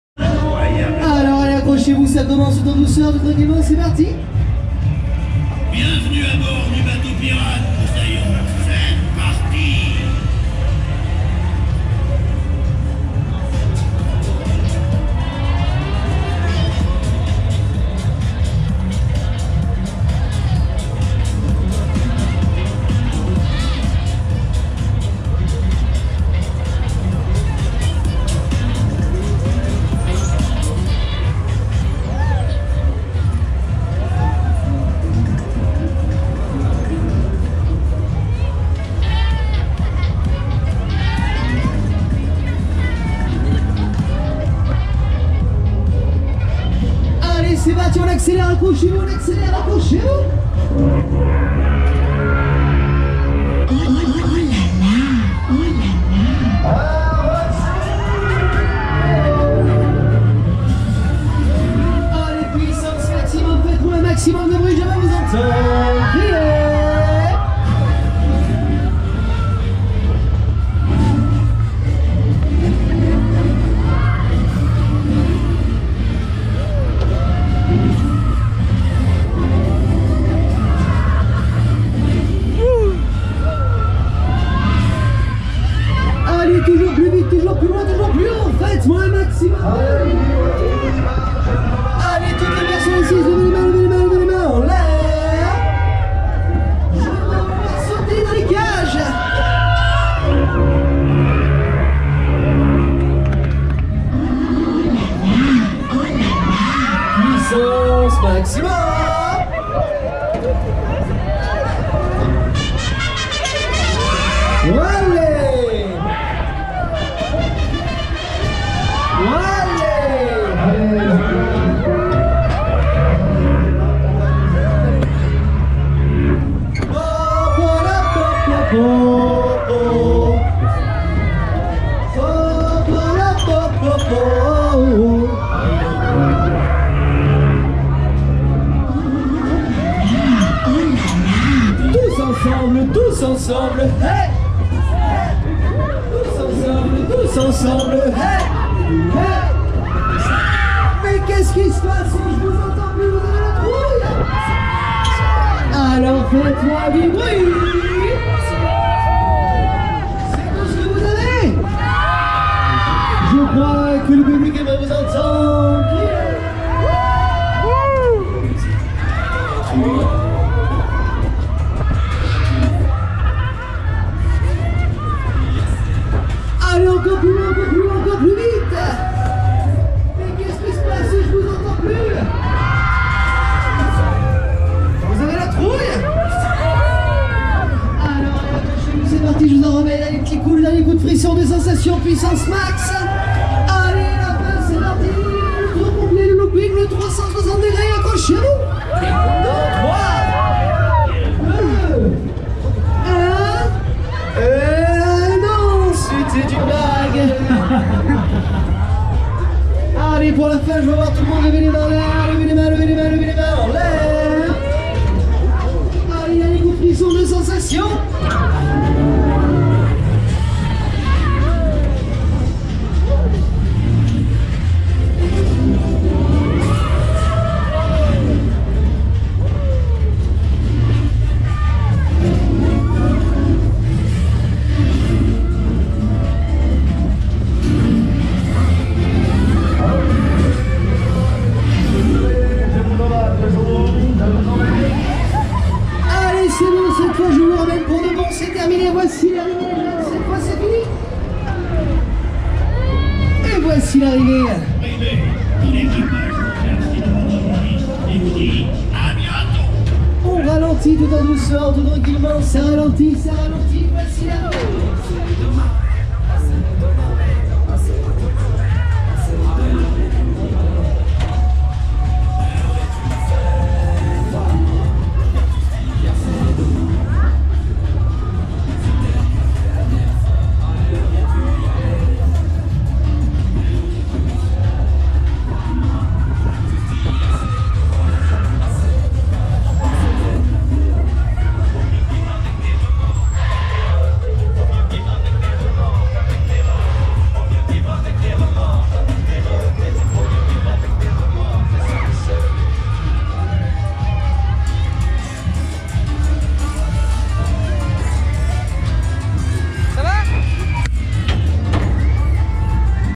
black pearl 2019
le black pearl de luna park de vichy 2019 monté a bort de black pearl :) avec capitaine jack sparrow bon voyage :)